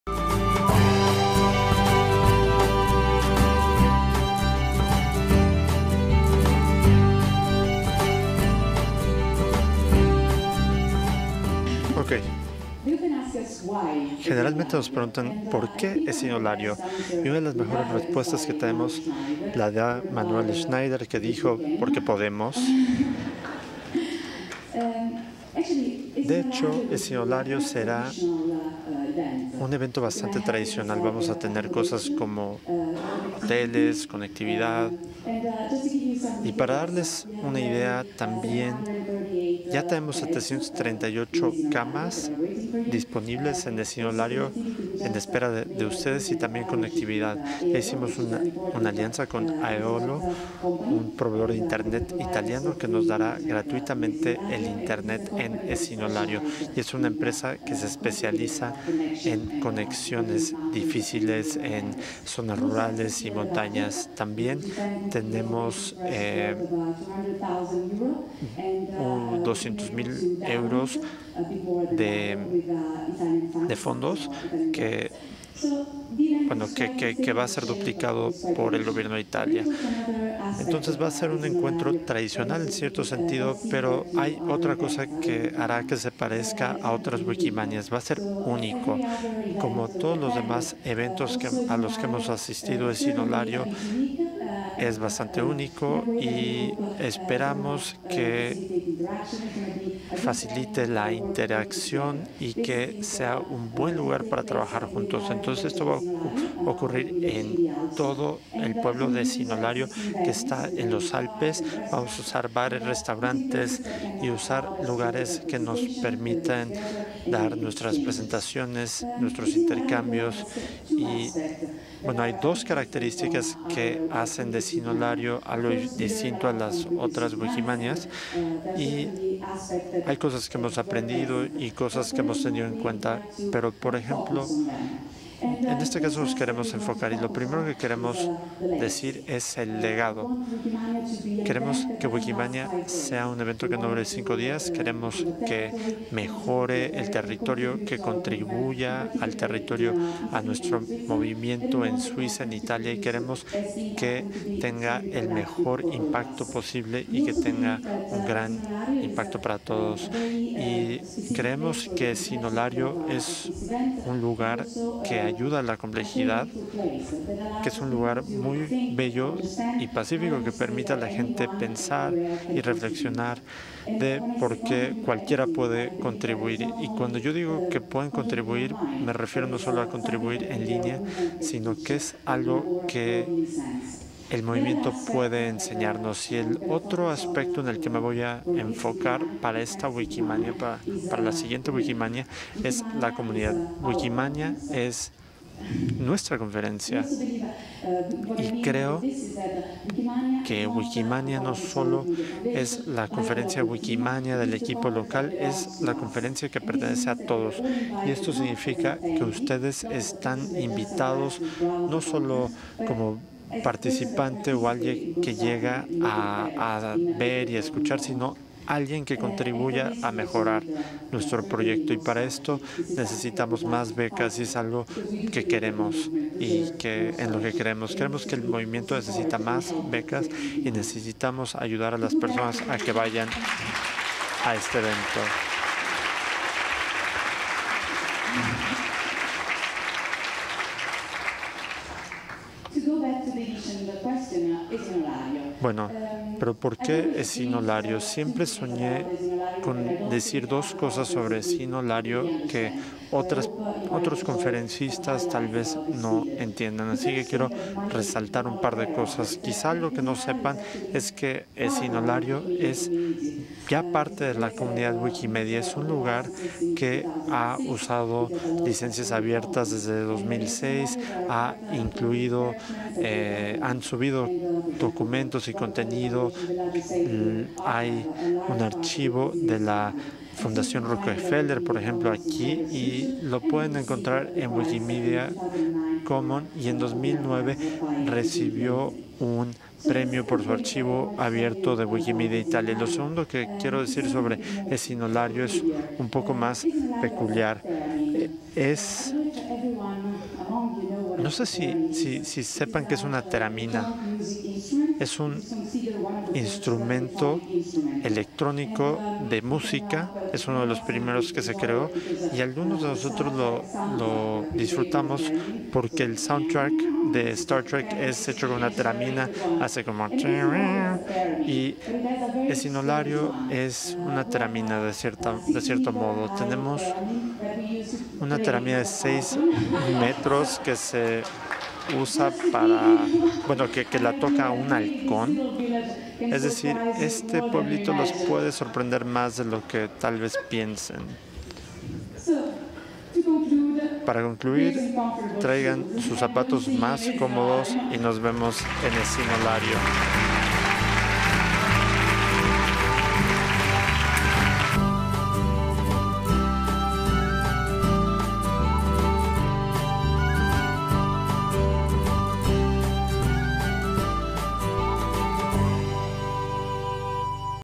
Wikimania 2015 - Presentación Esino Lario (Español)
Presentación de Wikimanía 2016 por Iolanda Pensa.

Video producido por Isla y Wikimedia México A. C.

Wikipedia, los proyectos de Wikimedia, Wikimania y el logo de Wikimania 2015 son marcas registradas por Wikimedia Foundation, Wikimedia México A. C. los usa con permiso de Wikimedia Foundation.